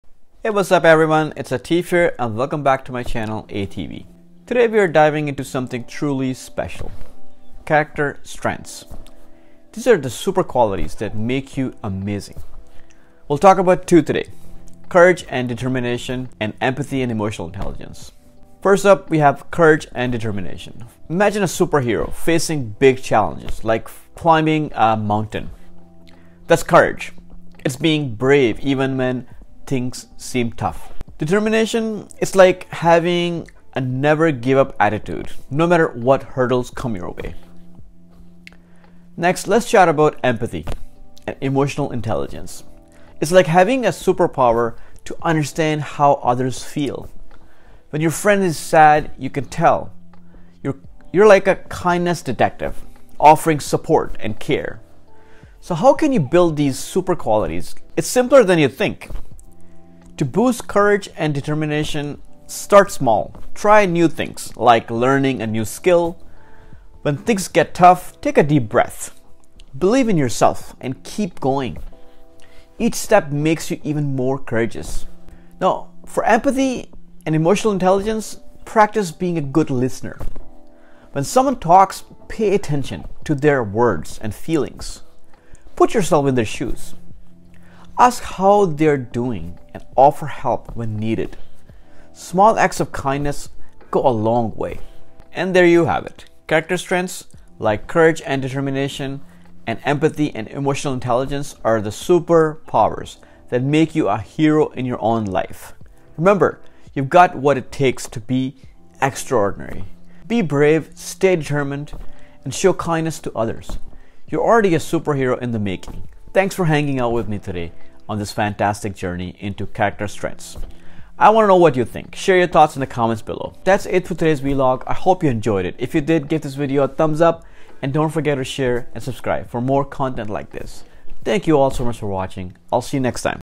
Unlocking Your Superpowers: Courage, Determination, Empathy, and Emotional Intelligence
Join me on a journey into the world of "Character Strengths." In this vlog, we explore the super qualities that make us extraordinary: Courage, Determination, Empathy, and Emotional Intelligence. Discover how you can cultivate these superpowers and become the hero of your own life. Remember, you've got what it takes!